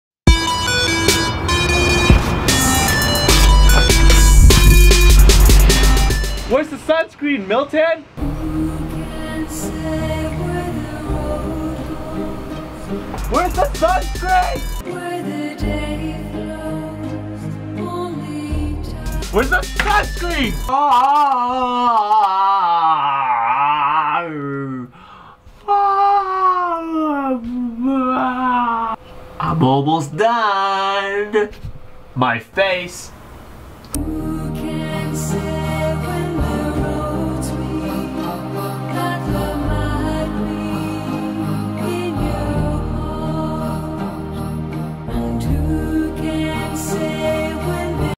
How to Apply Sunscreen
Get ready this summer and apply sunscreen with no shame!

Glenn Somera as Co-worker @obsoletecyborg
Milton Ng as Co-worker @miltonngtv